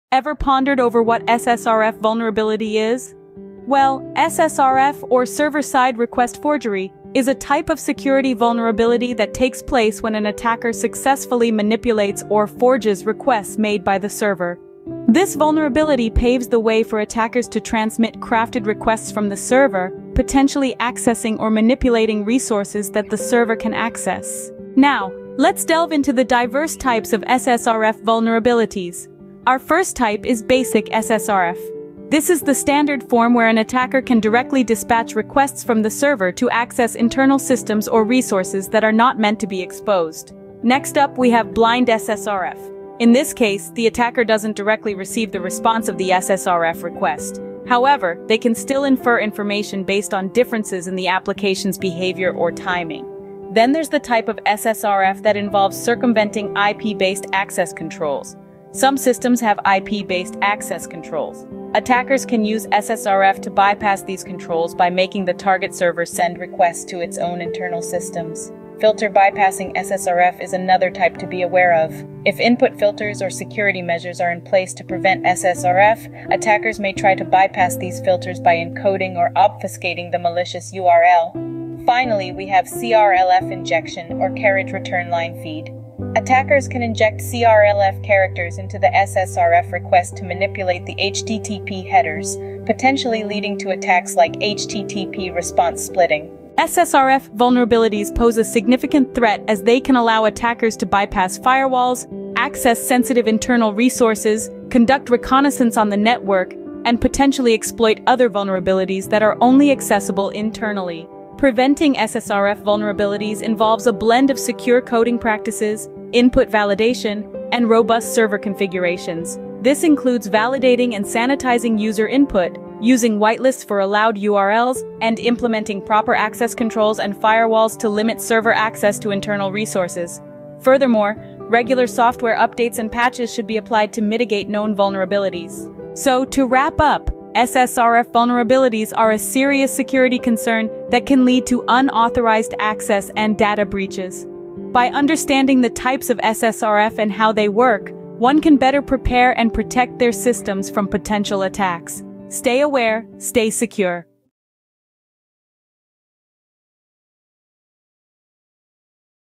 Unmasking SSRF: Understanding Server-Side Request Forgery
Welcome to our video where we dive deep into the concept of Server-Side Request Forgery (SSRF)! In this informative and educational video, we will explain what SSRF is, discuss its various types, and most importantly, unveil effective techniques to prevent such vulnerabilities.

SSRF can be a critical security concern for websites and web applications, as it allows malicious actors to manipulate server requests, leading to potential data breaches and unauthorized access. By understanding the intricacies of SSRF, you'll gain crucial knowledge to safeguard your online platforms and protect sensitive information.

Our expert presenter will simplify complex technical jargon, making it easier for you to grasp the essence of SSRF and apply preventive measures. With real-world examples, practical tips, and step-by-step demonstrations, this video is an invaluable resource for developers, security enthusiasts, and anyone interested in fortifying their online defenses.

Don't miss out on this engaging and enlightening exploration of SSRF! Together, we can build a more secure digital landscape!

OUTLINE:

00:00:00 Introduction to SSRF Vulnerability
00:00:24 Types of SSRF Vulnerabilities
00:01:36 Impact and Consequences of SSRF Vulnerabilities
00:02:15 Conclusion